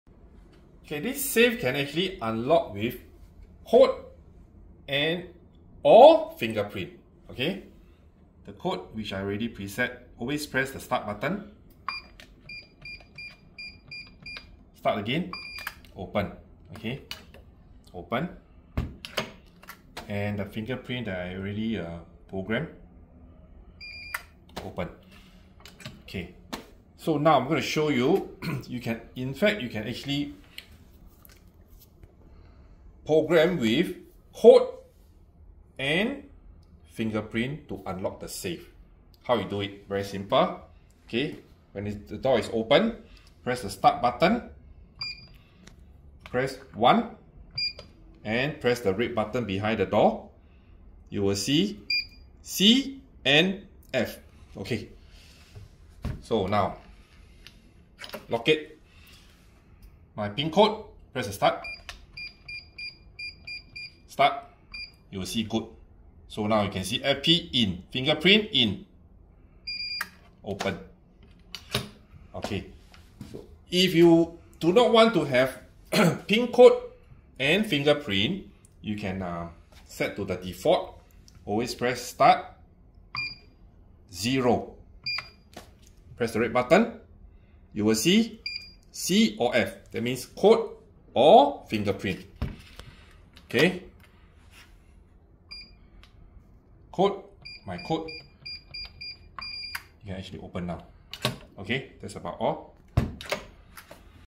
Yale Stellar/580/DB2 XL Fire Resistance 🔥Unlock with Pincode + Fingerprint
Yale Stellar 580/DB2 XL Fire Resistance Safe for Home and Office -
New Launch - Ready Stock in SG - Free Delivery - 1 Year Local Warranty!

The New Yale Stellar 580/DB2 XL Fire Safe, suitable for home and office, is a smart and highly convenient solution for keeping valuables safe. Your important belongings will stay safe and secure, whether at home or work.

With a range of options built with your security and satisfaction in mind, whether it's your camera to capture those special moments, your money for the next big holiday or a sentimental belonging, we have the right safe for you.

Secure and quick access: Enjoy the convenience of easy opening thanks to the built-in fingerprint sensor and keep your belongings safe -store up to 30 fingerprints.

Programmable electronic keypad: Stellar Safe offers a programmable electronic keypad that offers quick setup and easy operation. Enter the passcode using the keypad, then turn the knob to open the safe door.

Modern design to suit your home: Plus modern functionalities like USB backup in case of battery discharge and LCD keypad display for quick access.

Backup key: The safe includes an emergency backup key, which can be used to open it in an emergency. The key allows you to open the safe directly and access stored items.

You can conveniently access your belongings with your Biometric Fingerprint scan, personalized PIN code or an emergency manual key. (Come with 2 over-ride keys)

External Dimension: 580mm(H) x 400mm(W) x 400mm(D)
Internal Dimension: 540mm(H) x 360mm(W) x 320mm(D)
Net Weight: 40Kg
Volume: 60L